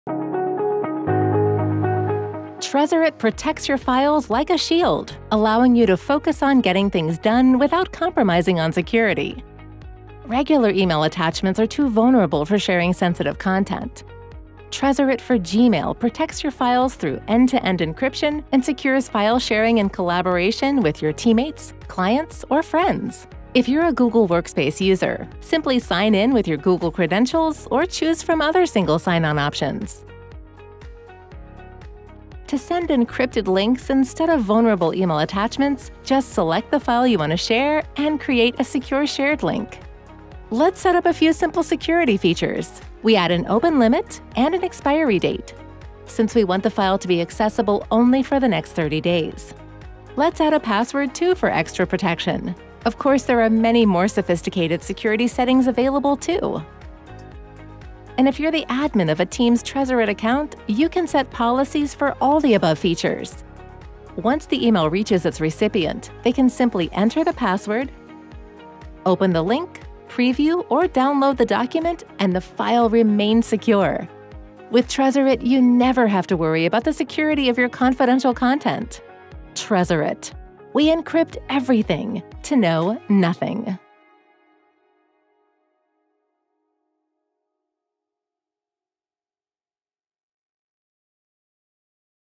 Tresorit for Gmail: End-to-end encrypted protection for shared files
Don’t compromise on security when sharing sensitive documents with teammates, clients, or friends. Tresorit for Gmail lets you encrypt email attachments, allowing you to focus on getting things done without compromising on security.